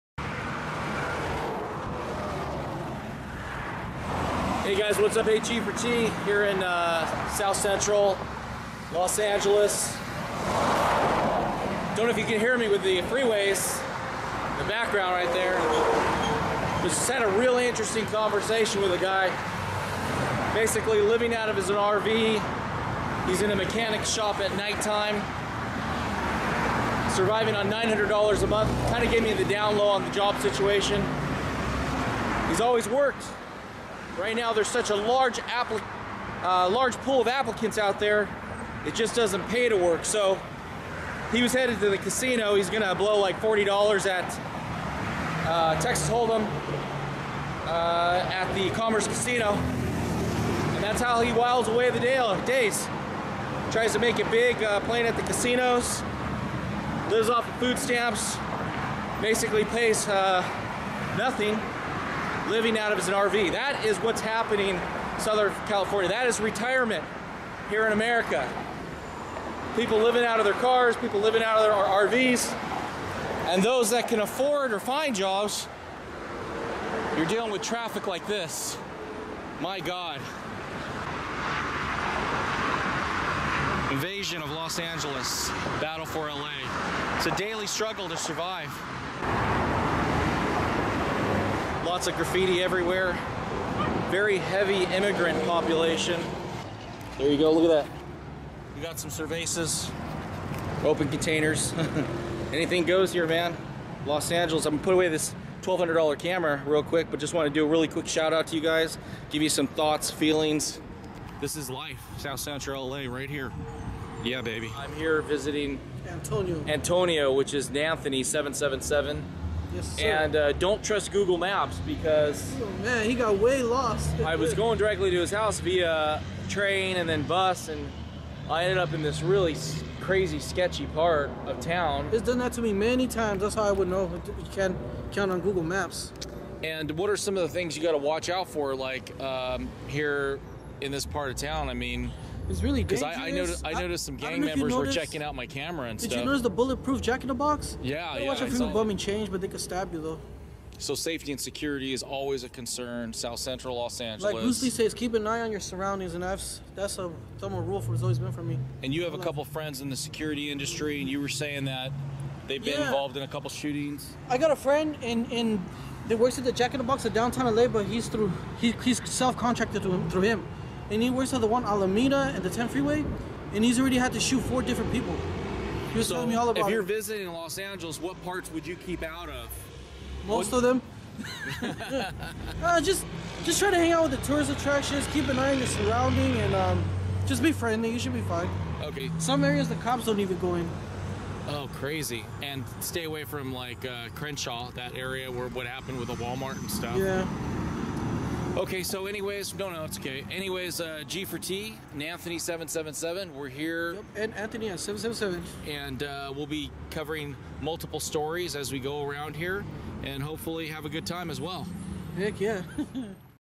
The Collapse of Los Angeles Life is Being Destroyed in LA G4t Nanthony777
In this video, George 1st talks about how he just talked to a mechanic who lives out of an RV and makes $900/mo.  He lives on food stamps and tries to hit it big at the casino playing Texas Holdem.  At the end of the video, George meets up w/ aka Antonio aka NAnthony777.  They end up talking about how to survive on the mean streets of Los Angeles.